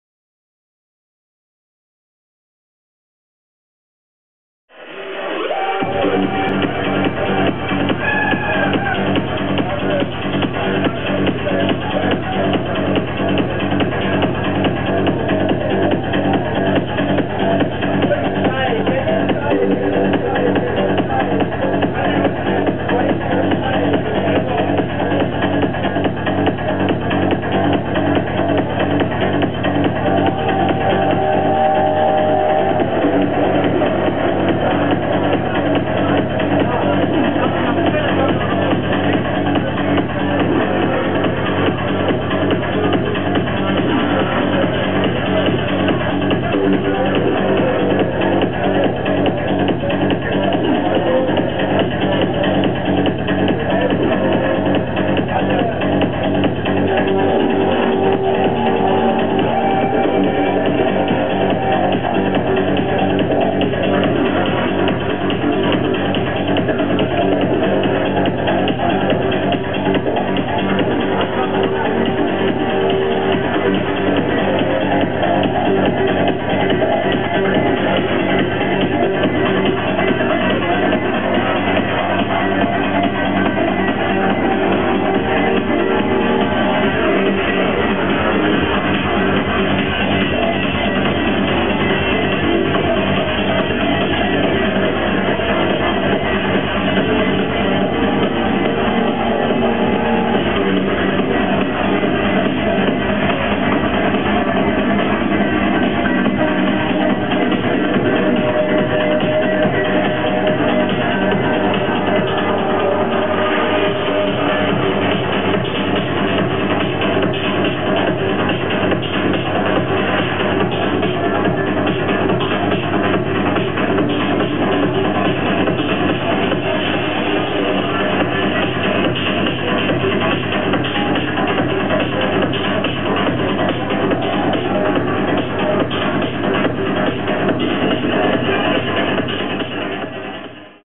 Psy-Kost Goaparty
Psy-Kost
Psytrance Party at "E35" in Leipzig / Germany
29. January 2010
Deco: Psytrancecamp